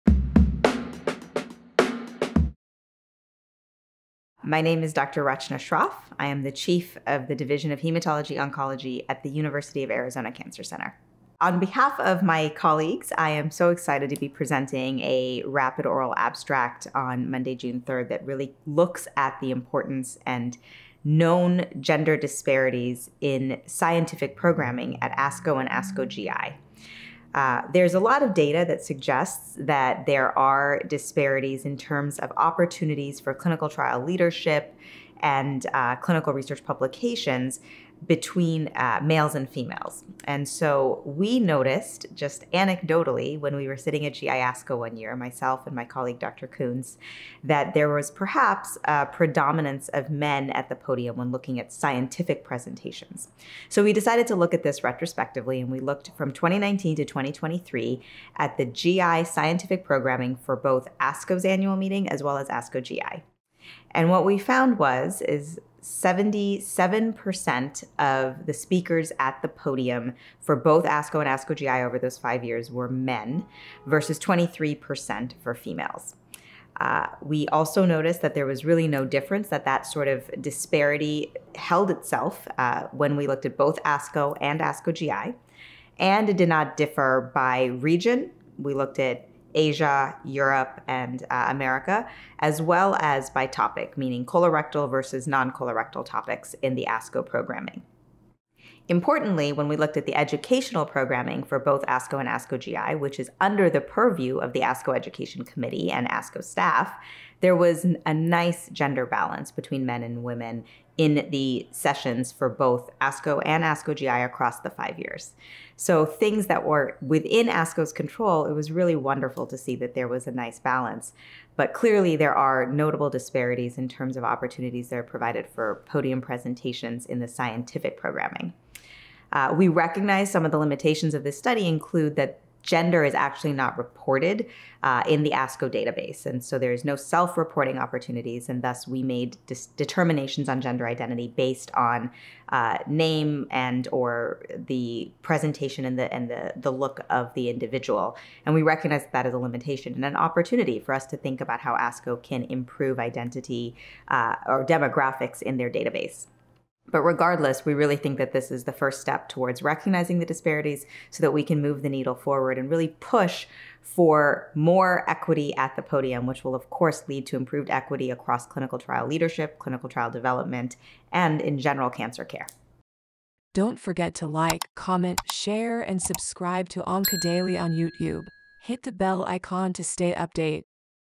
Is Gender Inequality Still Prevalent in Oncology?
Rachna Shroff, MD, MS, Chief of the Division of Hematology and Oncology at the University of Arizona Cancer Center, presents their study on the persistent issue of gender bias in oncology, highlighting findings and implications for the field.